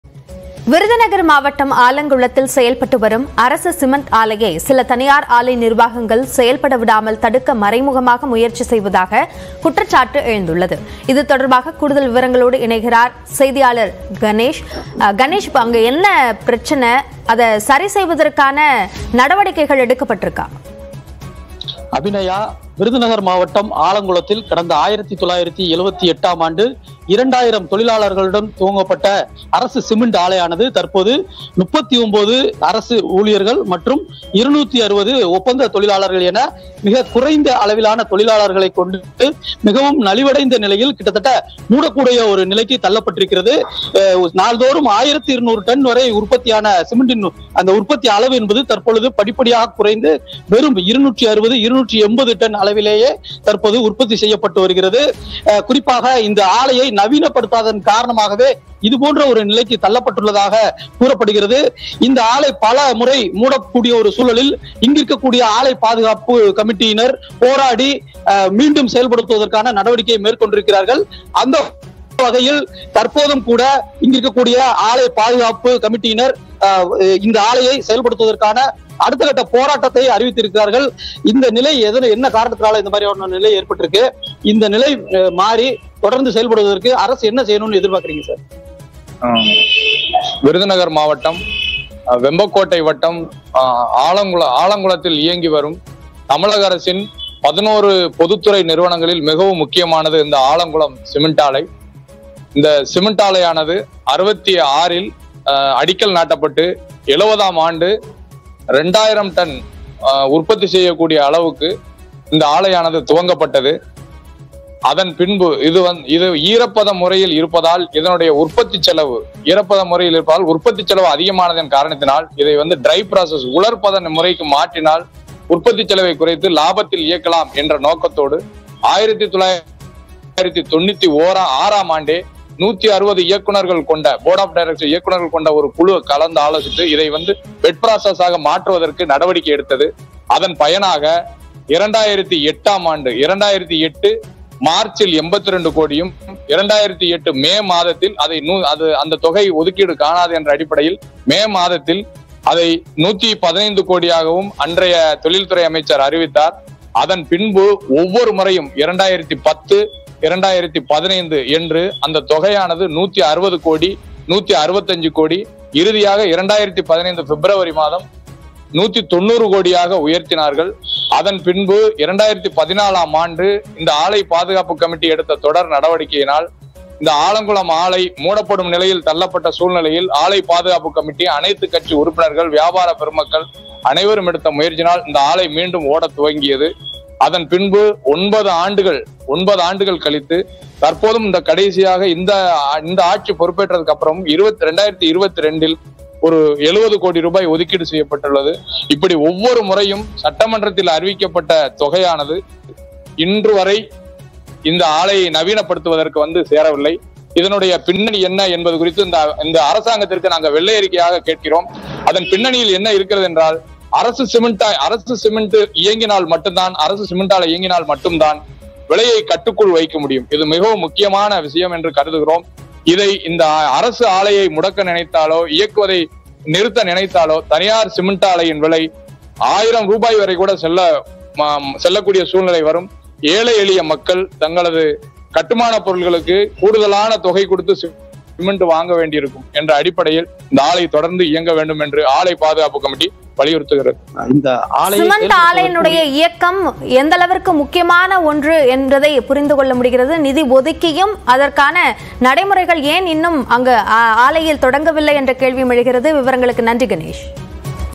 தனியார் நிறுவனத்தின் சதி செயல்..? புதிய சிக்கலில் சிமெண்ட் ஆலை - நடவடிக்கை எடுக்காதது ஏன்?
10 ஆண்டுகளில் 175 நிரந்த தொழிலாளர் எண்ணிக்கை 39ஆக குறைப்பு . சில தனியார் நிறுவனங்கள், அரசு ஆலையை முடக்க மறைமுக முயற்சி?.

list=PLZjYaGp8v2I8Jl8PfHo2MQdWKs42-15cX